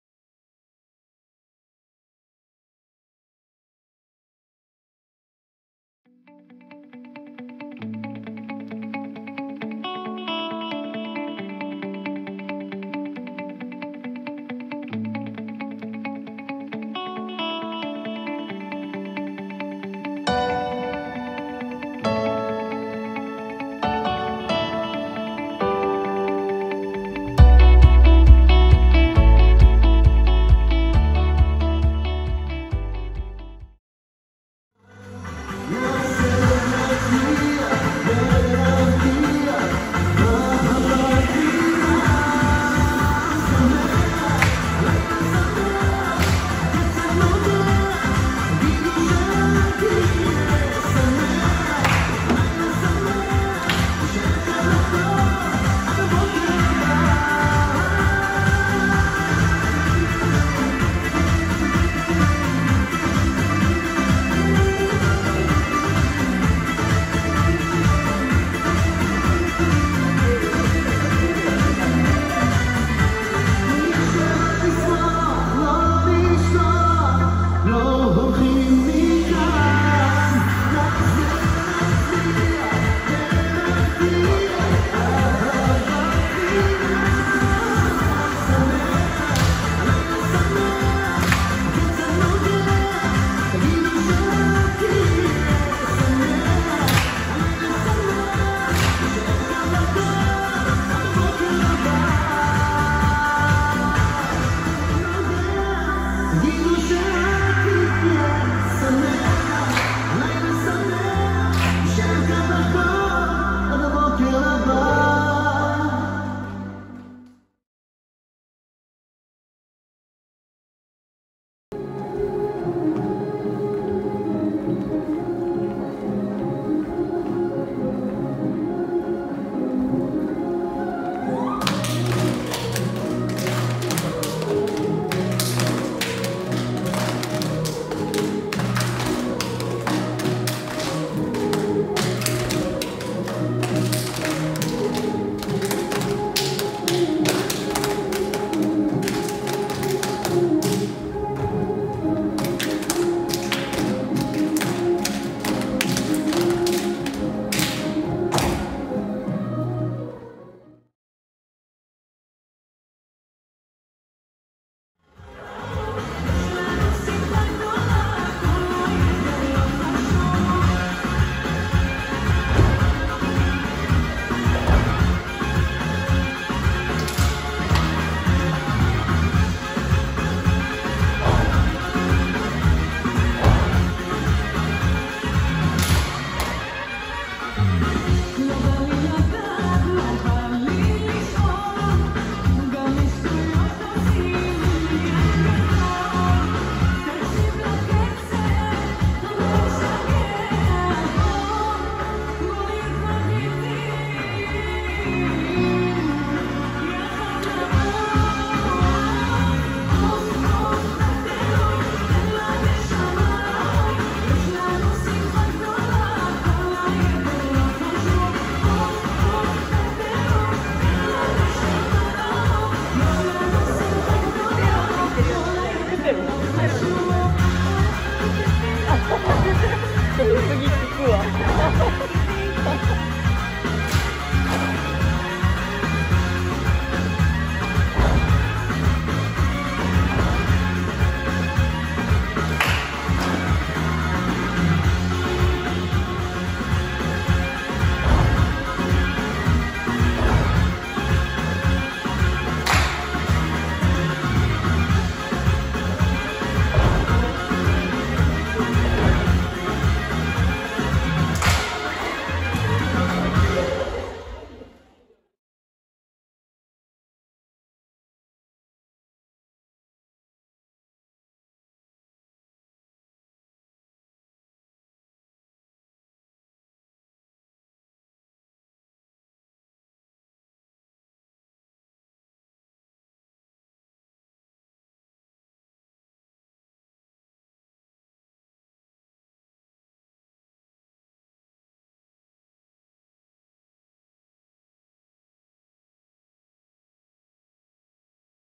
千葉大学民族舞踊部
2021年11月6日（土）から千葉大学webホームカミングデー2021を開催します！
千葉大学民族舞踊部の活動を紹介します！是非ご覧ください！